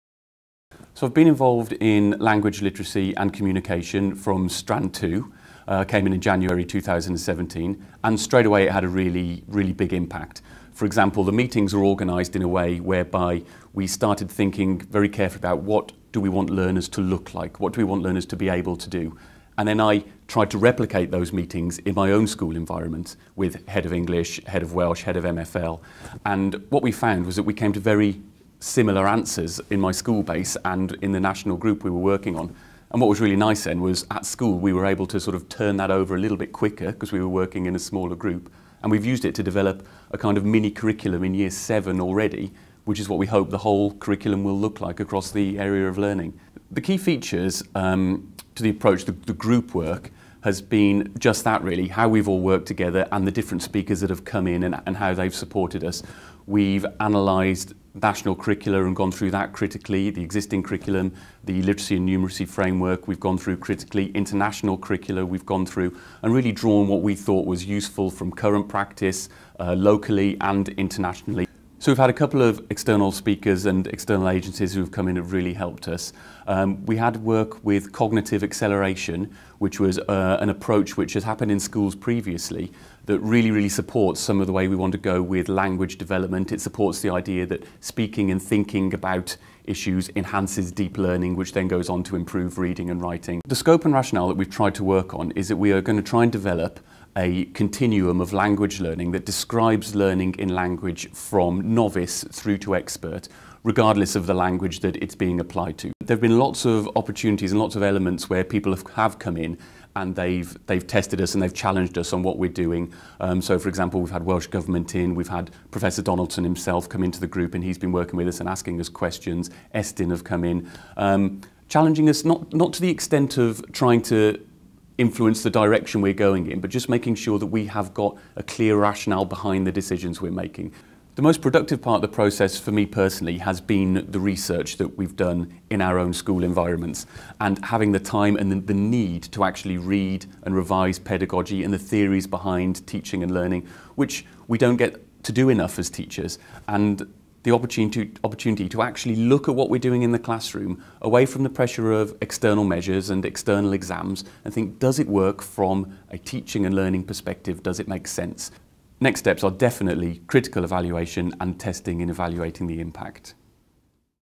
Matthew Bourne - Literacy, Languages and Communication Area of Learning and Experience
Developing a new Curriculum for Wales – teacher Matthew Bourne, Bassaleg School, talks about progress with Literacy, Languages and Communication